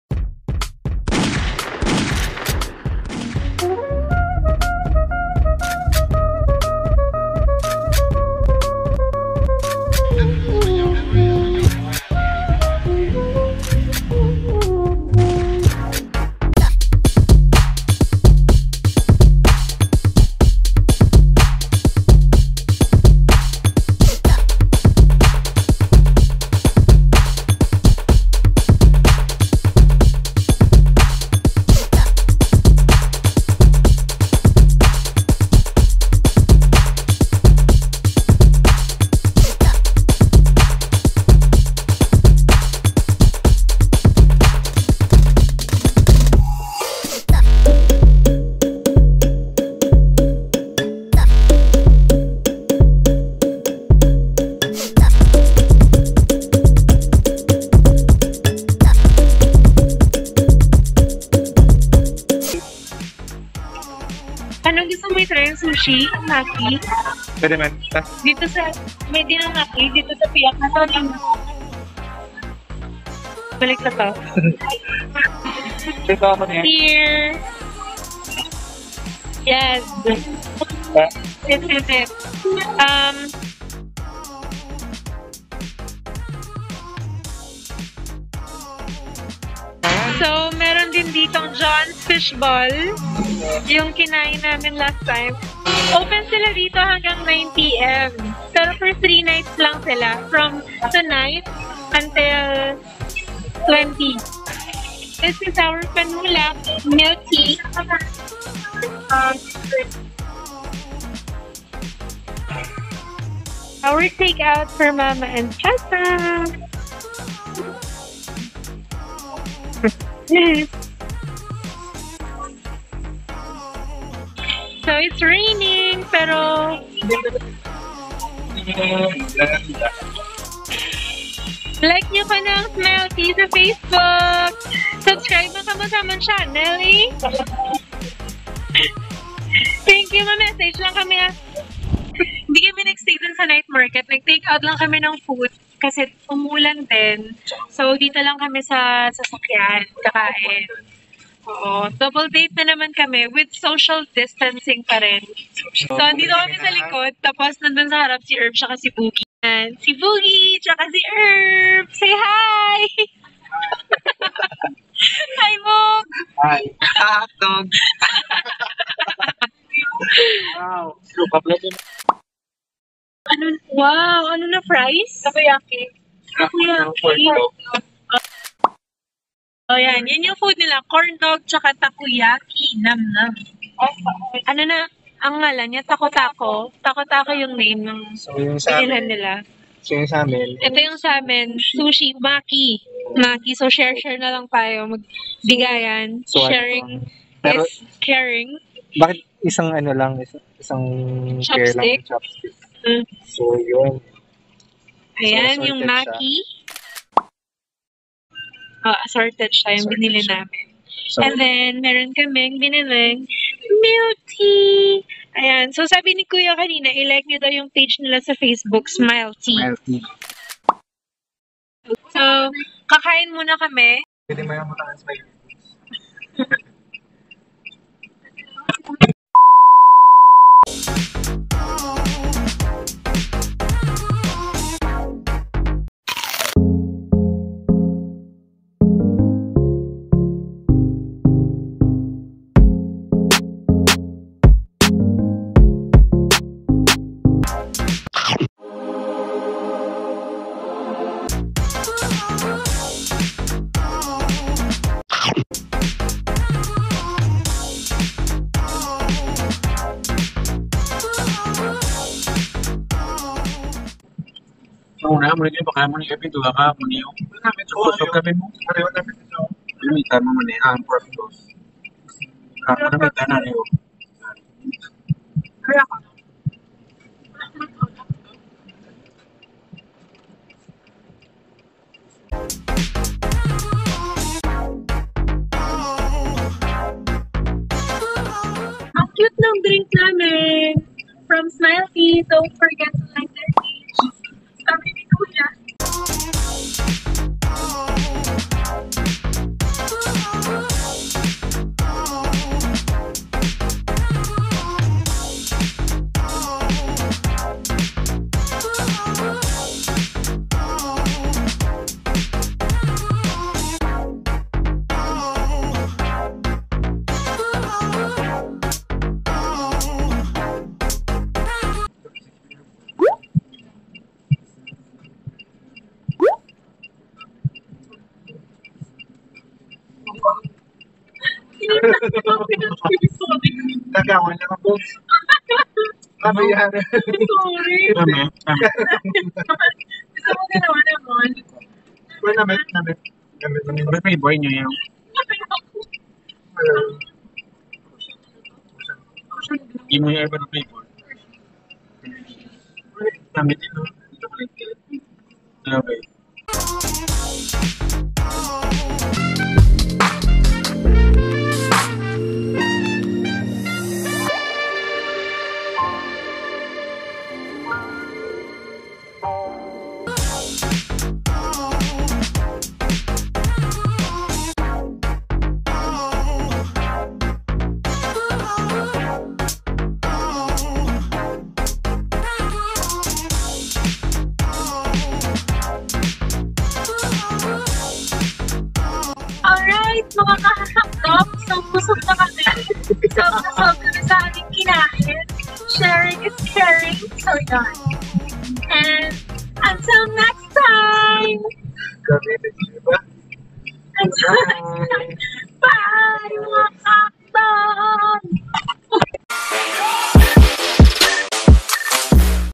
ILOILO CITY NIGHT MARKET + MUKBANG | TIN & TJ VLOGS
Another raw vlog, another aya ni Boogy sa amin na mag foodtrip 😂 at dahil on the go lang kami, yes naman gora lang basta busog 😁

Pumunta kami sa Night Market located at Iloilo Times Square, Gaisano Center and we bought food from:

then we ate in the car, so we decided to do another mukbang 😄

Enjoy watching and visit the Night Market, their last night is on December 20! ❤️

𝗥𝗮𝗱𝗶𝗮𝘁𝗶𝗻𝗴 𝗹𝗼𝘃𝗲 𝗮𝗻𝗱 𝗵𝗮𝗽𝗽𝗶𝗻𝗲𝘀𝘀,

𝑻𝒊𝒏 & 𝑻𝑱 ♥️